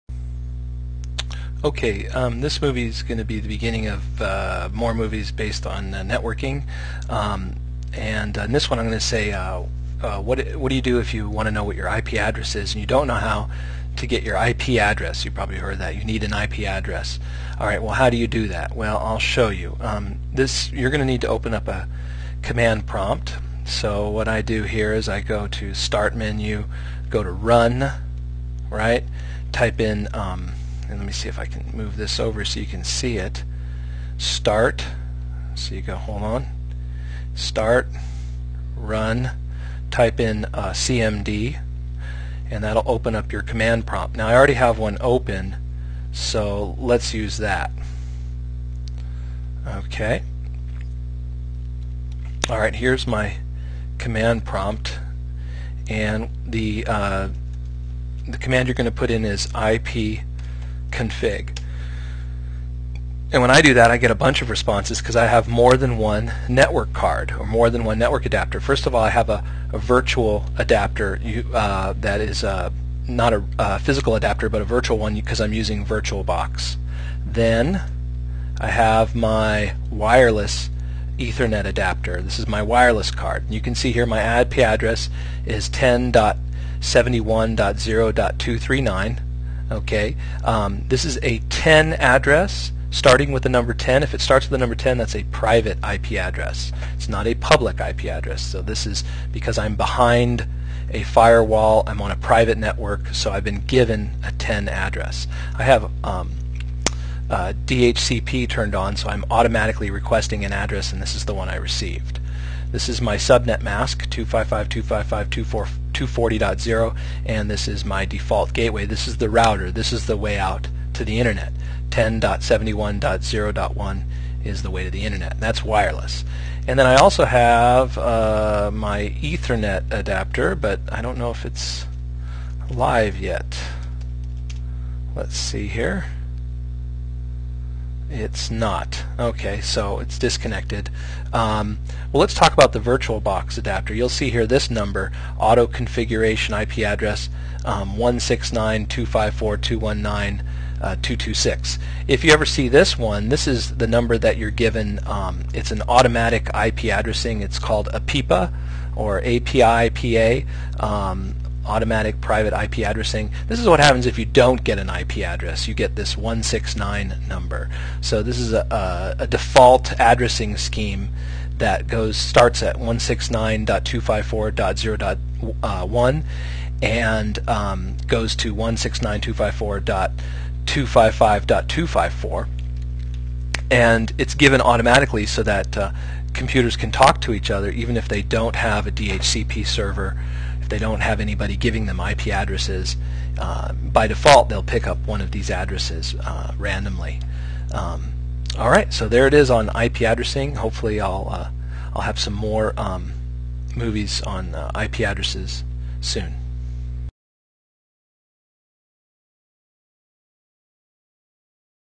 How do I find my ip address?
- A quick tutorial on finding out what your ip address is